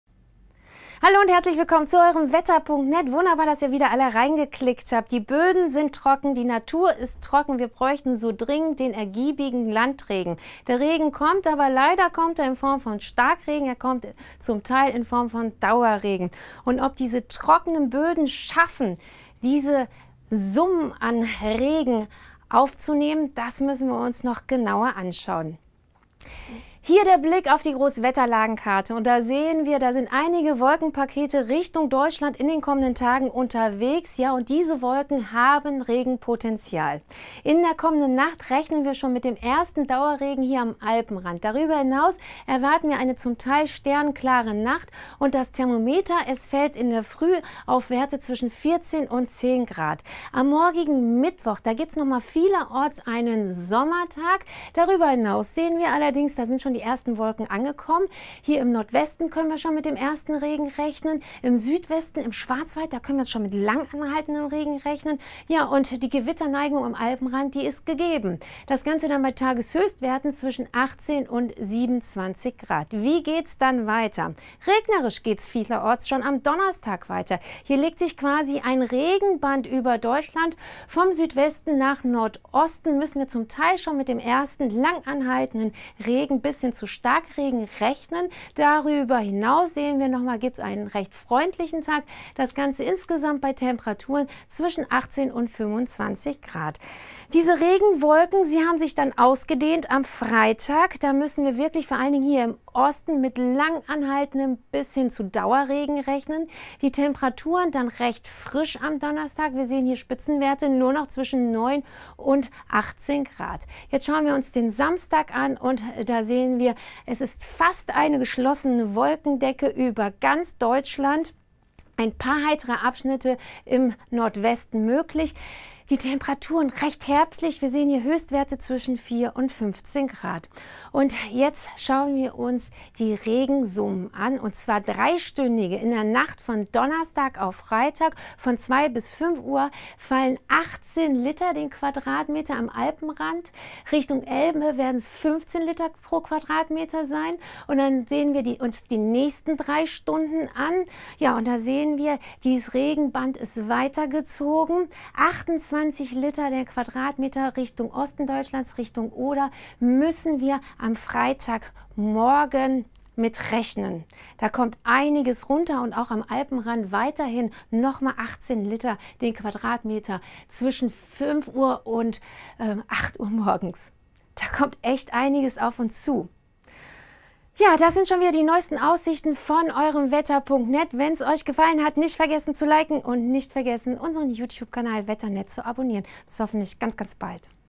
September-Wahnsinn: Freitag mit Temperatursturz! Extreme Regenmengen im Süden und Osten! Hochwasser!
In der Nacht auf Mittwoch schauert es im Süden örtlich, Mittwochnachmittag auch häufiger, lokal gewittrig. Sonst ist es heiter und warm. Die Nacht auf Donnerstag wird windig. Tief Wicca sorgt ab der Nacht auf Freitag und über das Wochenende im Süden und Osten für einen Temperatur- und Wettersturz. An den Alpen fällt bis nach Garmisch und Oberstdorf hinab bei 1 bis 4 Grad Schneeregen oder Schnee.

Das aktuelle Deutschlandwetter vom 22. September 2020.

Quelle Fotos: hans / pixabay